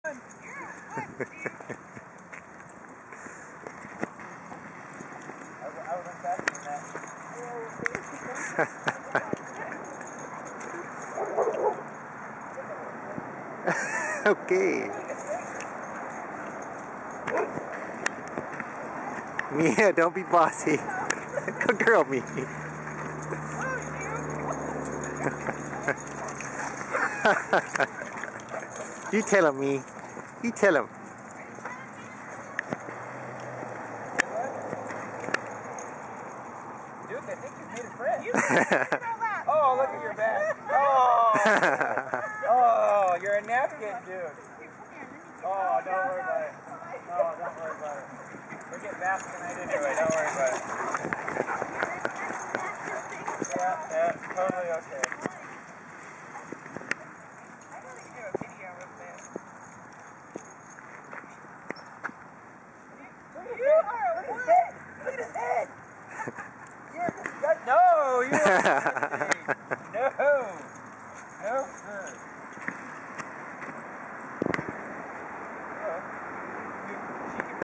At the Dog Park
Baylee, Simba and Mia at the dog park.